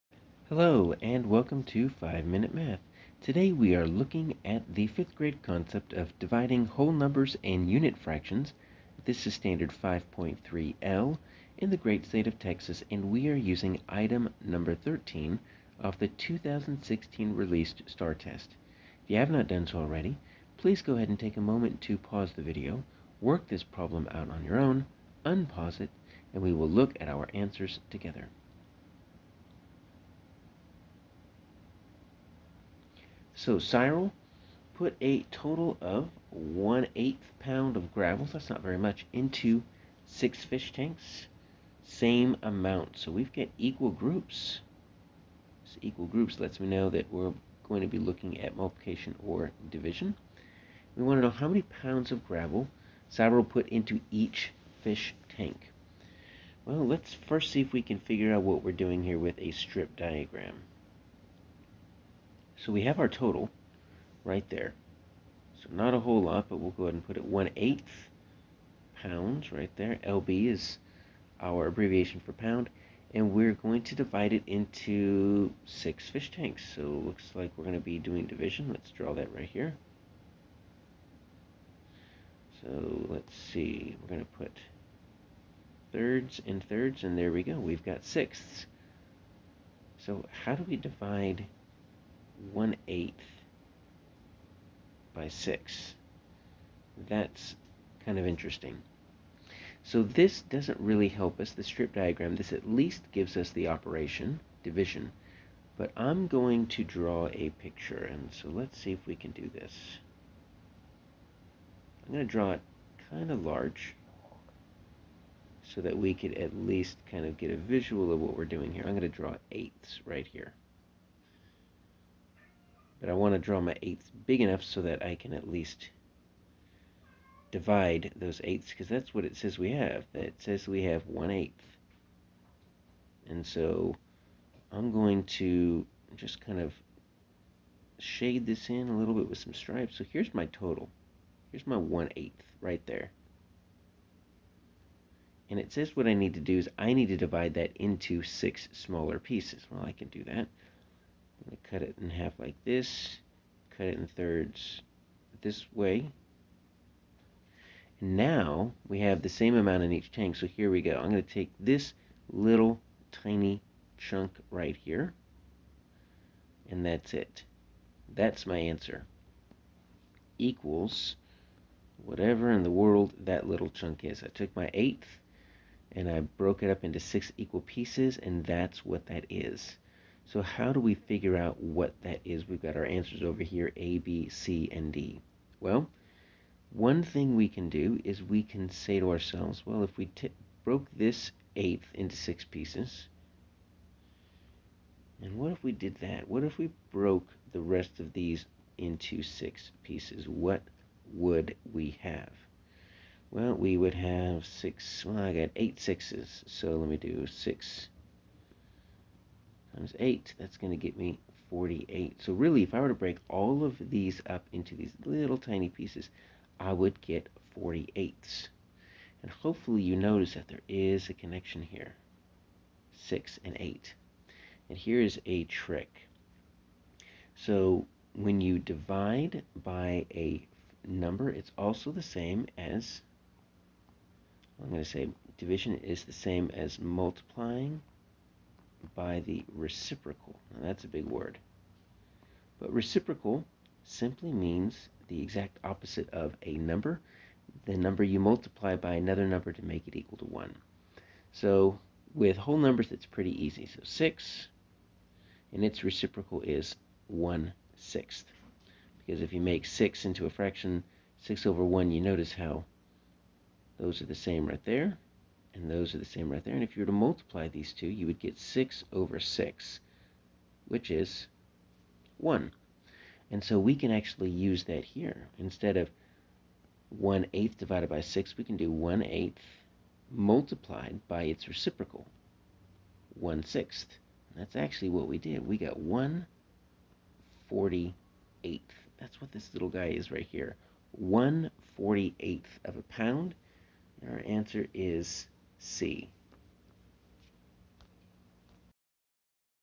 5th Grade STAAR Practice Dividing Whole Numbers and Unit Fractions (5.3L - #1)
Practice divide whole numbers by unit fractions and unit fractions by whole numbers with a test item from the 2016 released STAAR test (#13 - 5.3L).